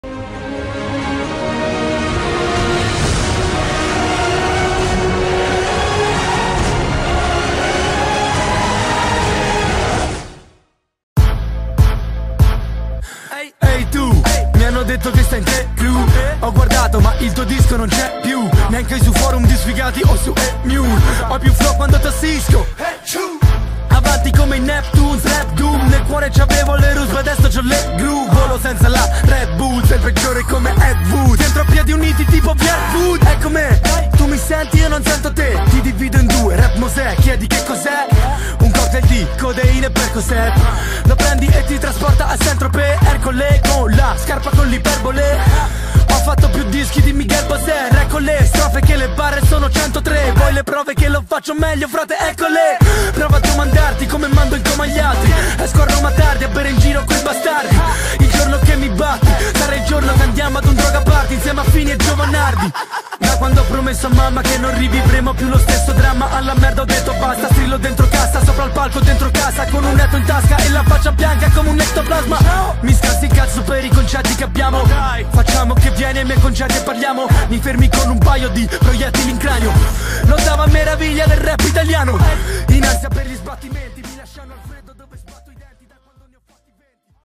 Introducing Storm Seem
Ciao a tutti ragazzi e oggi siamo qui con l'introducing del feeder Storm_Seem.

"Scusate per la bassa qualità video delle clip, pace!" (cit Pionz)